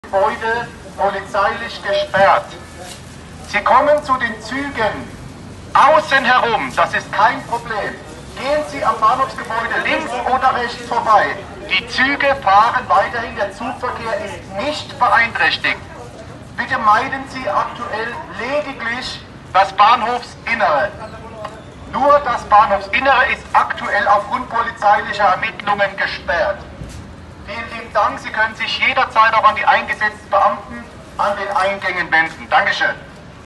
Corona-Verdacht: Mannheimer Hauptbahnhof wird gesperrt
Wegen eines Coronavirus-/Covid-19-Verdachtsfalles wird am Abend des 27. Februar 2020 das Eingangsgebäude des Mannheimer Hauptbahnhofs kurrzeitig gesperrt. Video: Timo Benss